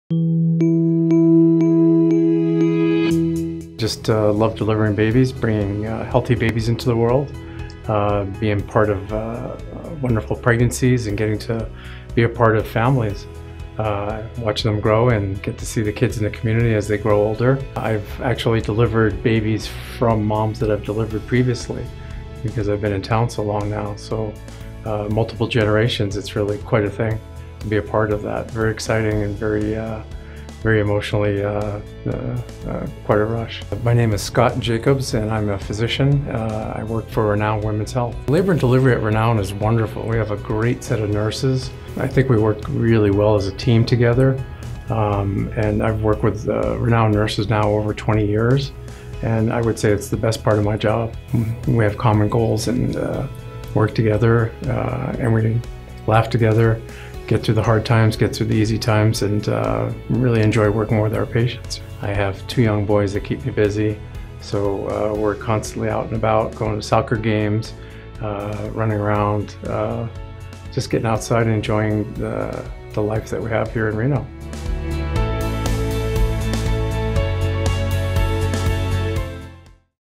Scott Jacobs, MD, Obstetrics & Gynecology
Dr. Jacobs enjoys bringing babies into the world and being part of wonderful pregnancies and families. Working with Renown nurses on Labor & Delivery for more than 20 years, he touts the collaborative and positive relationships that the nurses and doctors have says it's the best part of his job.